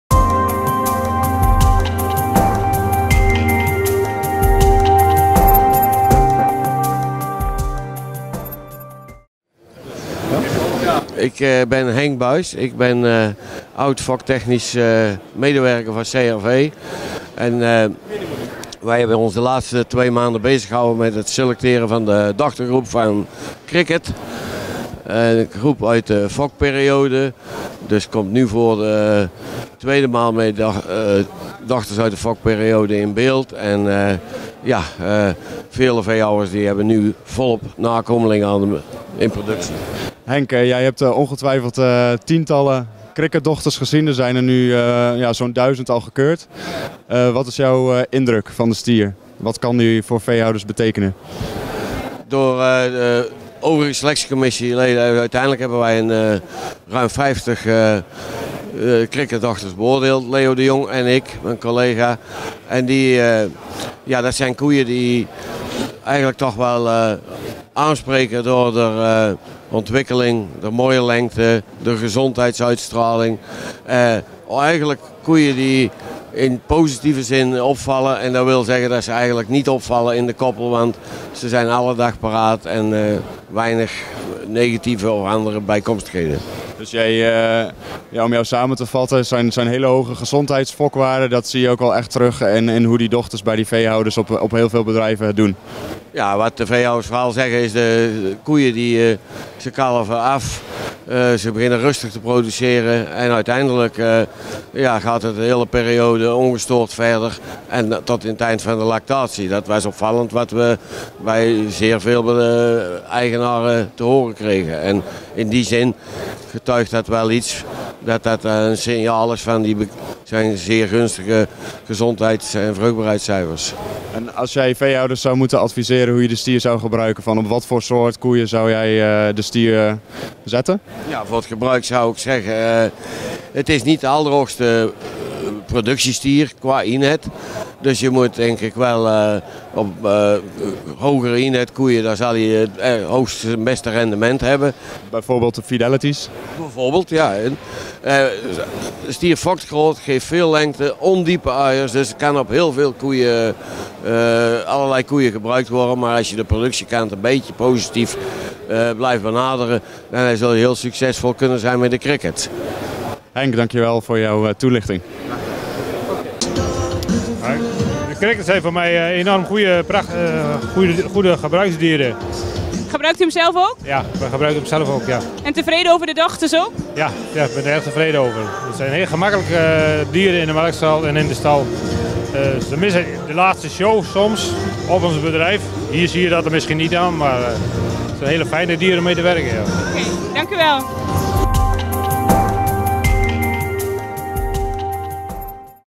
Experts aan het woord - dochtergroep Cricket
Wat vinden de experts van de dochters van Cricket? Tijdens de CRV Koe-Expo in Zwolle werden deskundige Henk Buijs (voorselectie) en een toeschouwer aan de tand gevoeld over de dochtergroep van deze stier.